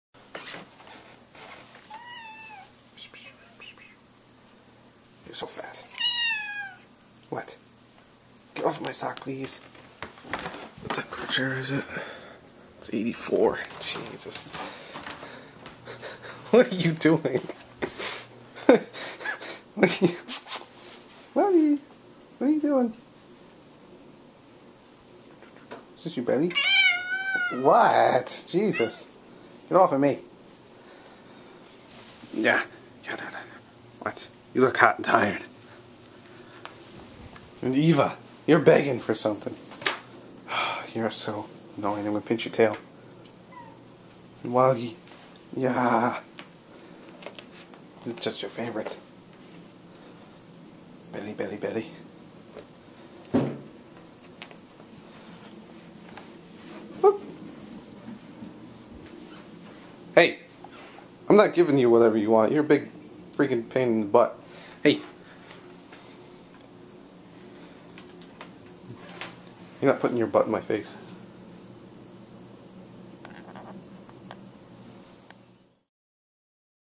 My Cats 5-29-2012
They were hot and didn't like it when I played with their tummy.